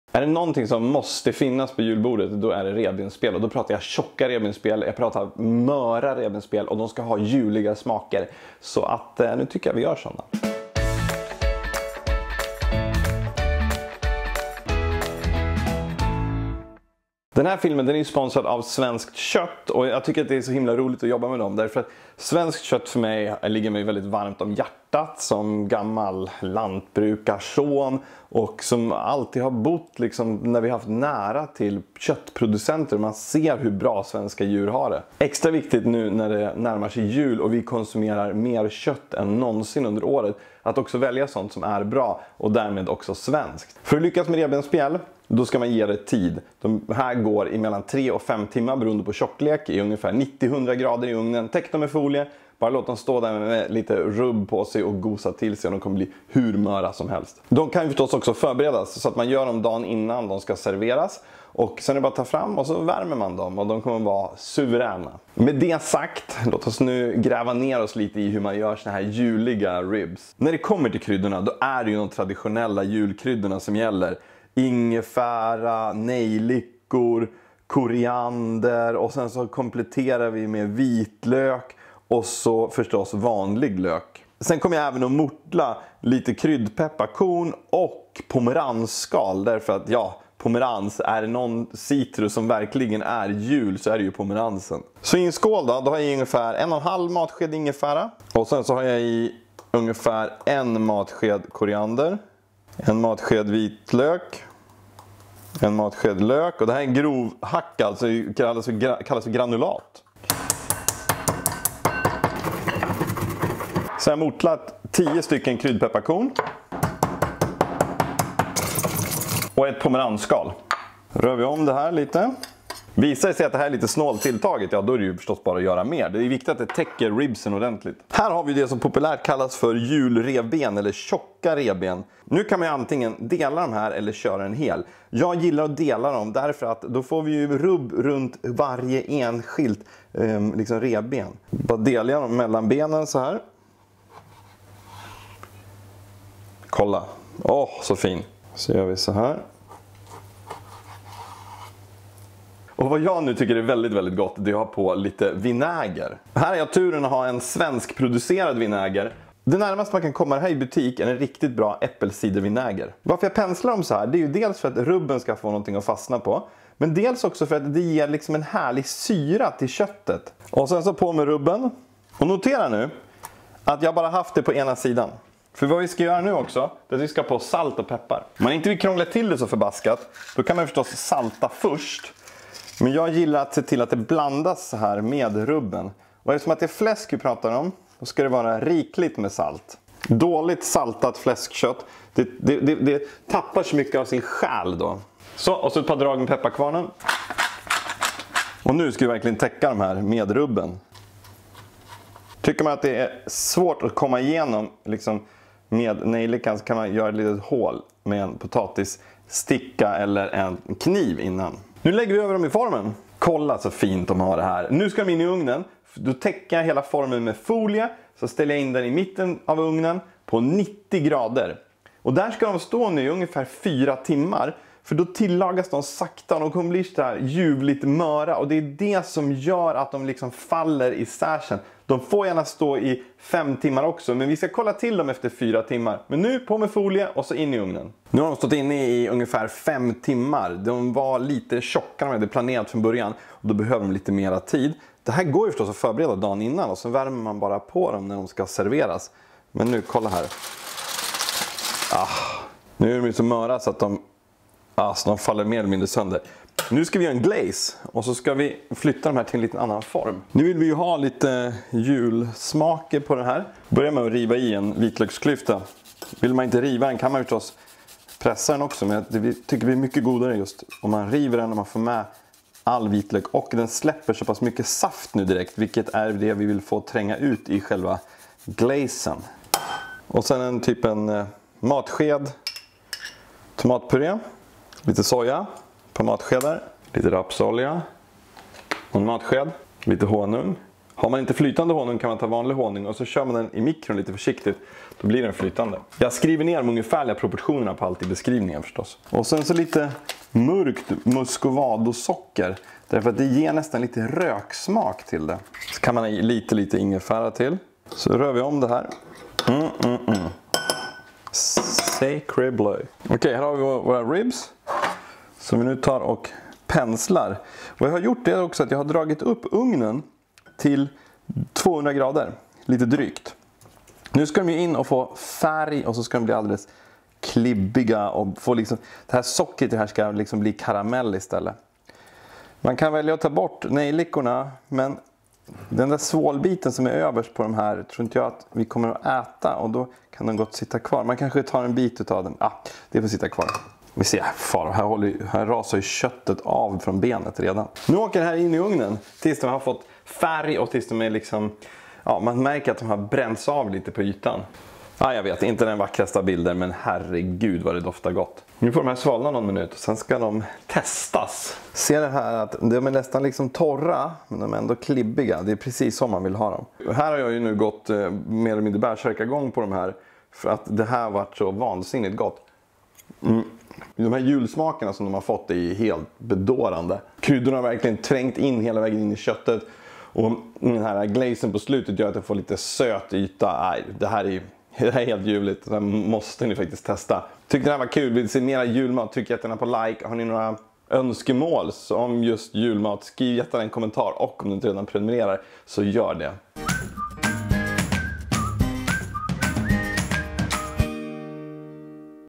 Till julbordet: Möra ribs med underbar smak!
Det här är ett säkert sätt att göra succé med ribsen i jul. Fulla med smak och faller sönder i munnen - precis som de ska vara.

Den här filmen är i samarbete med Svenskt Kött. Läs mer om tjocka revben och vad man kan göra med dem här:

Kryddblandning:
1.5 msk ingefära
1 msk koriander
1 msk vitlök
1 msk lök
10 mortlade kryddpepparkorn
1 mortlat pomeransskal
Salt och peppar
Hela nejlikor

Glaze:
1 vitlöksklyfta
1 msk tomatpuré
2 msk japansk soja
1 msk rapsolja
1 msk honung
2 msk mörkt muscovadosocker
Lite ingefära